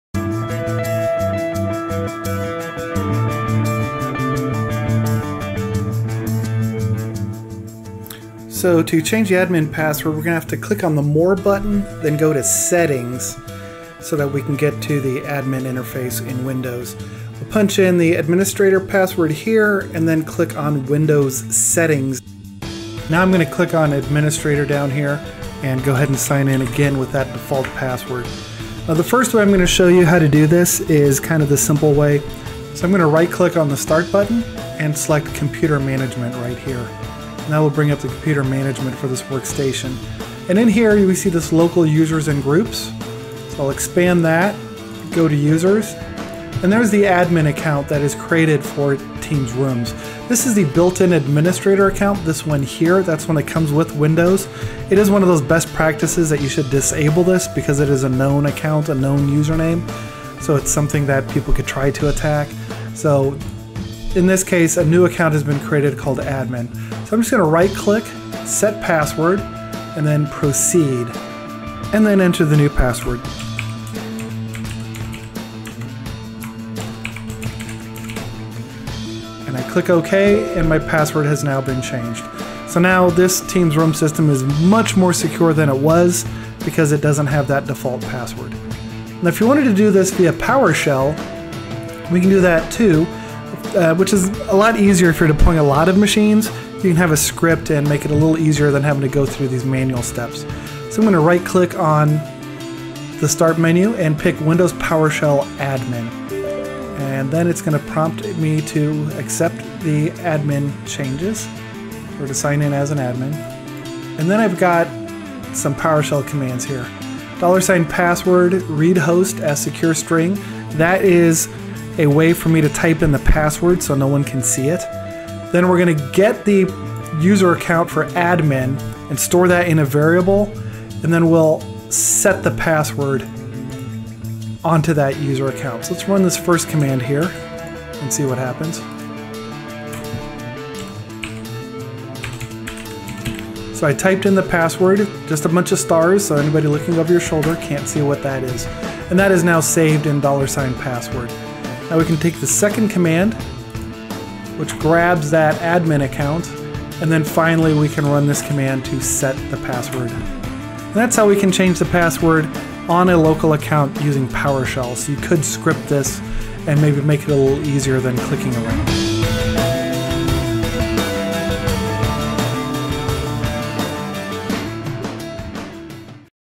Teams Room Quick Tips: Change Admin Password
Teams Rooms Quick Tips are a series of videos to help you get a better understanding of Teams Rooms by showing real-world examples of setting up, configuring, and using Teams Rooms.

In this video, I show how to change the default Admin password. This is the very first thing you should do after completing the out of box experience.